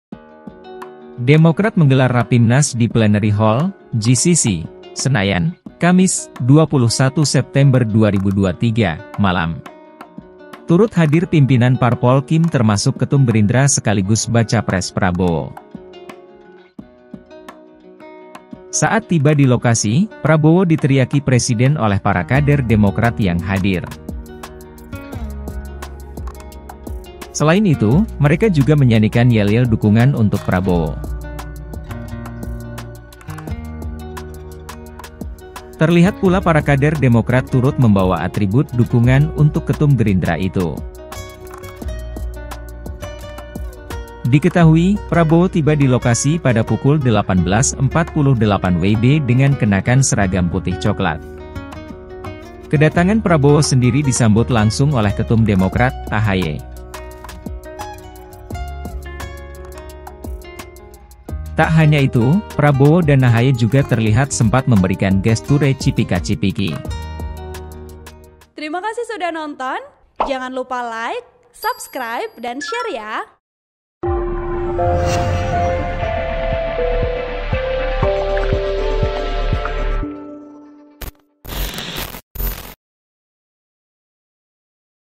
Momen Prabowo Diteriaki Presiden di Rapimnas Demokrat, Para Kader Lakukan Ini untuk Sang Menhan
TRIBUN-VIDEO.COM - Partai Demokrat menggelar Rapat Pimpinan Nasional (Rapimnas) di Plenary Hall, Jakarta Convention Center (JCC), Senayan, Kamis (21/9/2023) malam.

Dalam agenda tersebut turut hadir para pimpinan partai politik Koalisi Indonesia Maju (KIM), termasuk Ketua Umum Gerindra sekaligus bakal calon presiden Prabowo Subianto.

Saat Prabowo tiba di lokasi, seluruh kader Demokrat yang memadati kursi peserta hingga tribun atas terdengar meneriakkan seruan Prabowo presiden.

Tak hanya itu, mereka juga turut menyanyikan yel-yel dukungan untuk Prabowo Subianto maju sebagai capres.

"Yo, ayo ayo Prabowo, ku ingin kita harus menang, Prabowo Presiden," seru para kader Demokrat di lokasi.

Lebih lanjut, di lokasi juga terlihat para kader Demokrat turut membawa aribut dukungan untuk Prabowo Subianto.

Mereka membawa spanduk kecil hingga flyer kotak dengan tulisan Prabowo Presiden.

Sebelumnya, Ketua Umam Partai Gerindra sekaligus bakal calon presiden (capres) dari Koalisi Indonesia Maju (KIM) Prabowo Subianto hadir langsung dalam acara Rapat Pimpinan Nasional (Rapimnas) Partai Demokrat.

Berdasarkan pantauan Tribunnewscom di lokasi, Prabowo Subianto tiba sekitar pukul 18.48 WIB dengan mengenakan seragam putih, peci hitam dan celana panjang berwarna cokelat.

Kedatangan Prabowo Subianto itu sendiri disambut langsung oleh Ketua Umum Partai Demokrat Agus Harimurti Yudhoyono (AHY) di lokasi.

Keduanya terlihat saling bersalaman saat Prabowo turun dari mobilnya.

Tak hanya itu, Prabowo dan AHY juga terlihat sempat memberikan gesture cipika-cipiki.

Tidak banyak pernyataan yang disampaikan keduanya, Prabowo Subianto terlihat bersama AHY terlihat langsung memasuki pintu VVIP.

Prabowo terlihat hanya melambaikan tangan ke arah awak media yang sudah menunggu.

Tak hanya AHY, terlihat pula beberapa jajaran pengurus DPP Partai Demokrat.

Mereka seperti Wakil Ketua Umum Demokrat Edhie Baskoro Yudhoyono hingga Sekjen Demokrat Teuku Riefky Harsya.

Artikel ini telah tayang di Tribunnews.com dengan judul Yel-yel Prabowo Presiden Menggema di Lokasi Rapimnas Demokrat,
Editor: Adi Suhendi